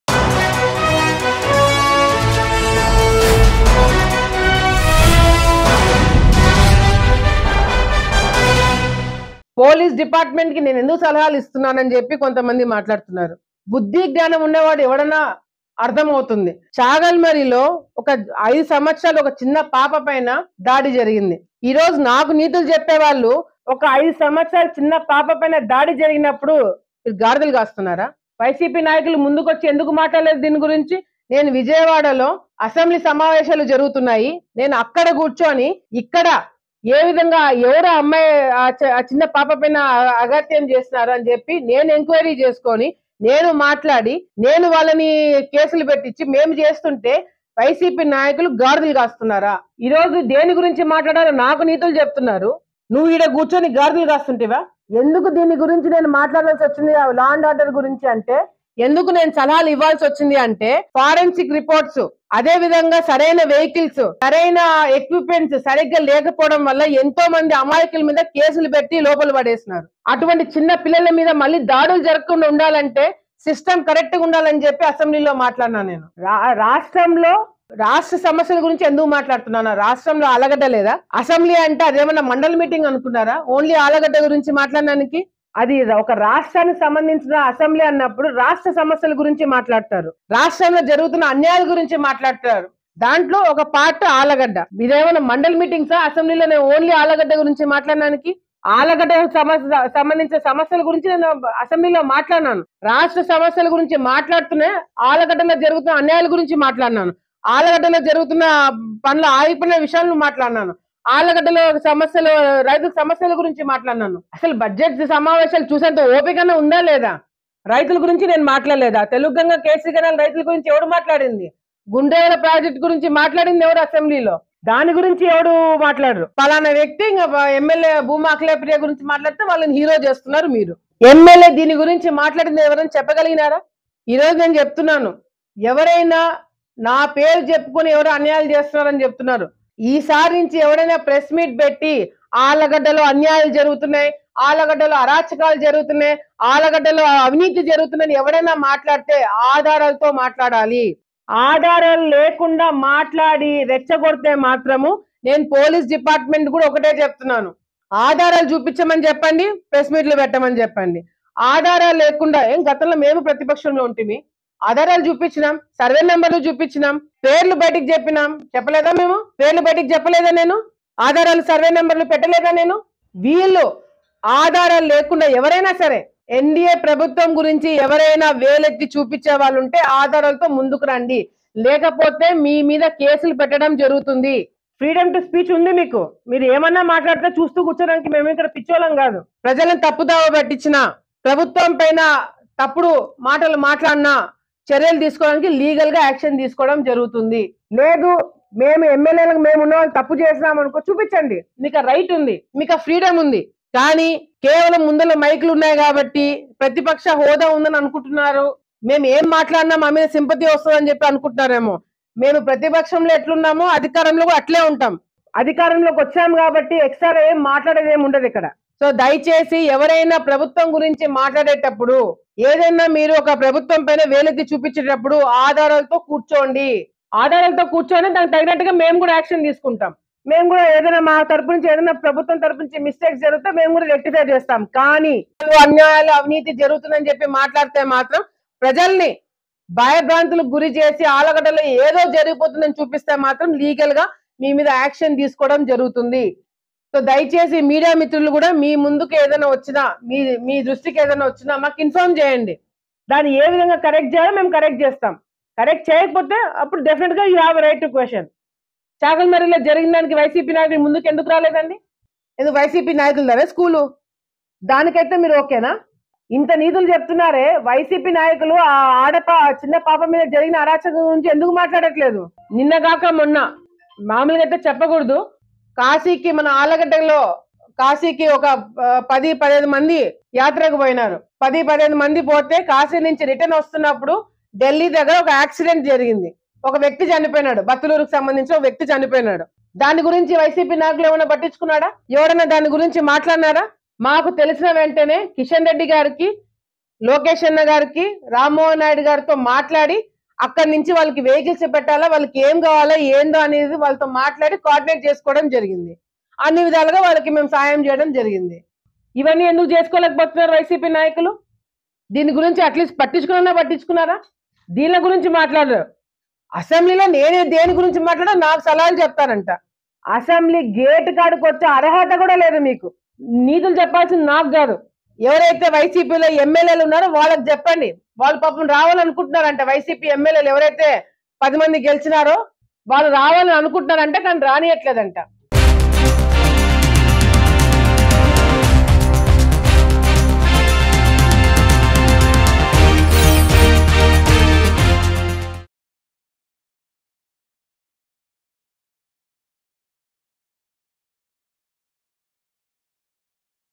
Bhuma Akhila Priya Aggressive Comments On YSRCP Leaders | TDP Vs YCP | AP Politics | Mango News
Bhuma Akhila Priya Aggressive Comments On YSRCP Leaders. She is calling on YCP leaders to present proof regarding the accusations against her.